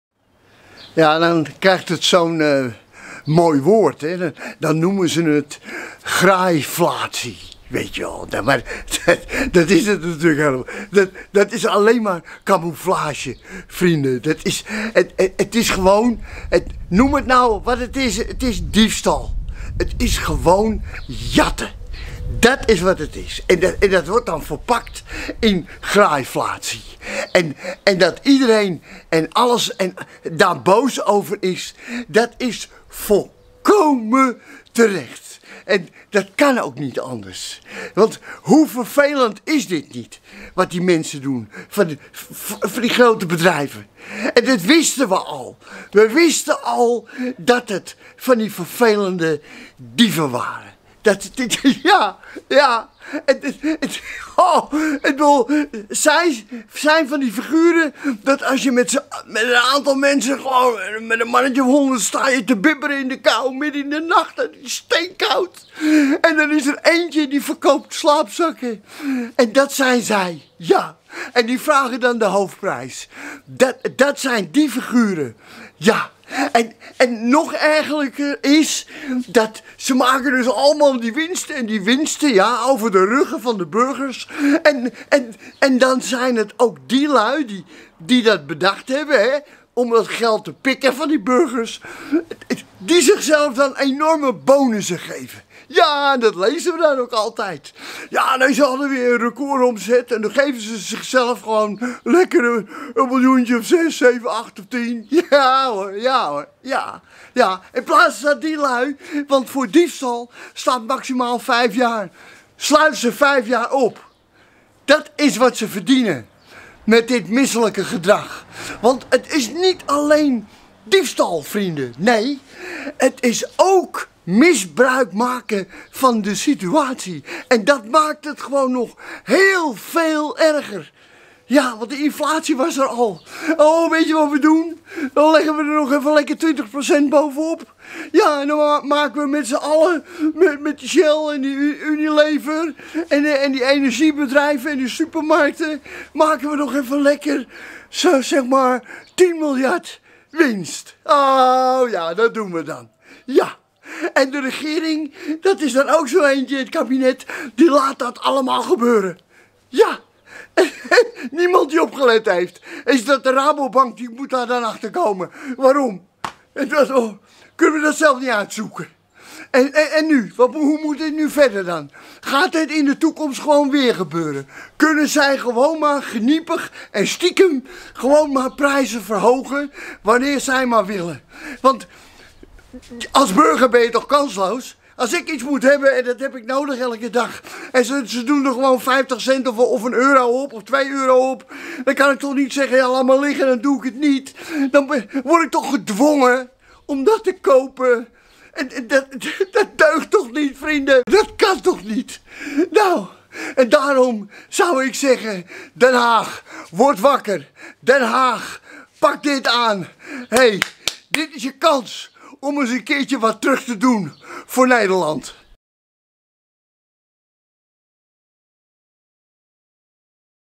Even over de graaiflatie!
Nieuwsbericht:
Onderzoek van economen van de Rabobank toont aan dat bedrijven hun prijzen meer hebben verhoogd dan de stijgende kosten rechtvaardigen. „Wanneer bedrijven hun verkoopprijzen niet verder hadden verhoogd dan minimaal noodzakelijk zou zijn geweest om hun winst op peil te houden, dan had de stijging van afzetprijzen in 2022 theoretisch gezien 2,2 procentpunt lager kunnen uitpakken”, concluderen zij in een onderzoek dat vrijdag verschijnt.